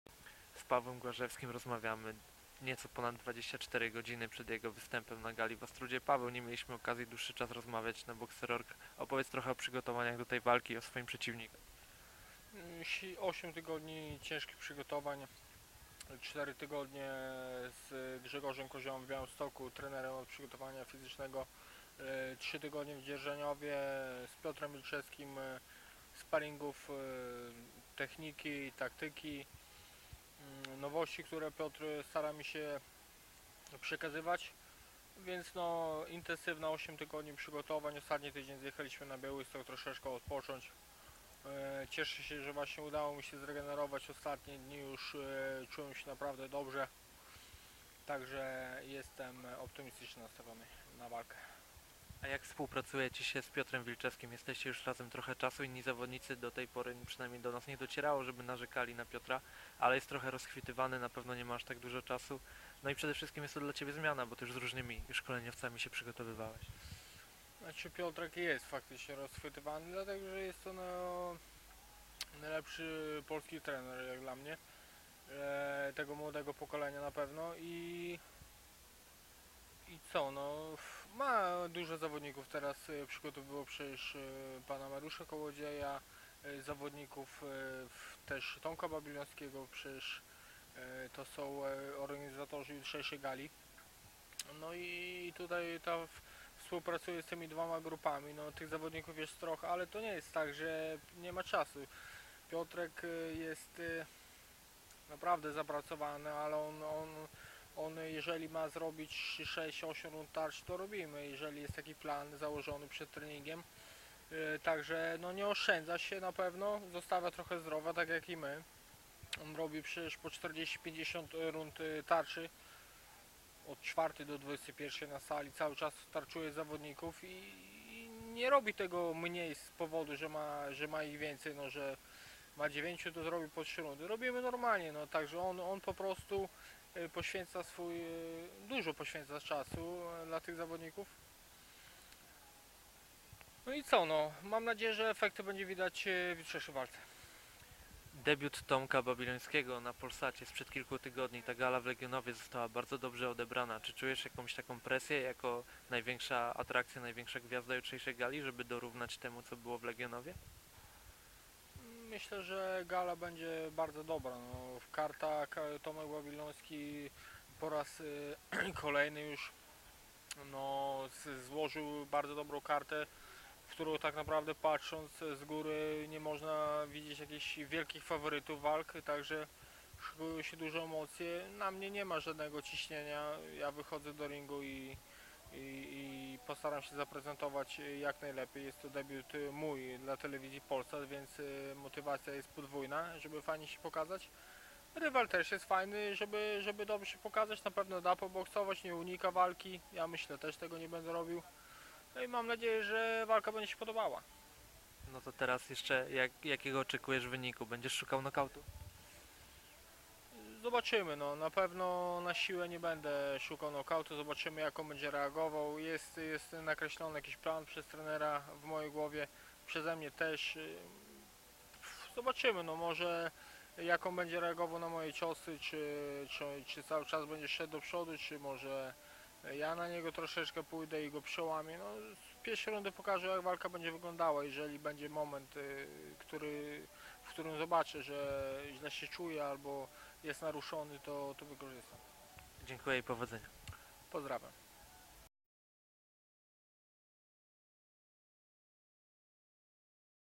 Paweł Głażewski przed walką na gali w Ostródzie
- Największy portal bokserski w Polsce przedstawia wywiad Leszka Dudka z Pawłem Głażewskim na nieco ponad 24 godziny przed występem "Głaza" na gali w Ostródzie.